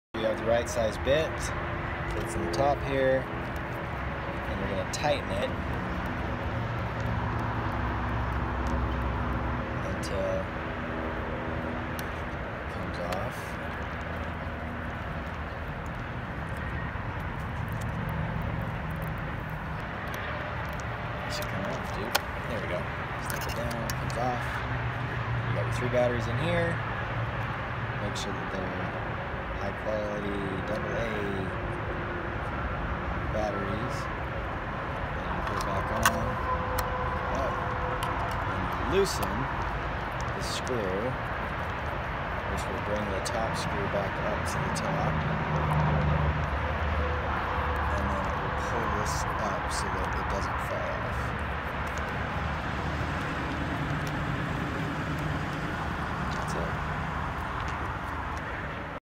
SALTO KS escutcheon crashbar battery replacement
How to change batteries in an escutcheon crashbar lock from Salto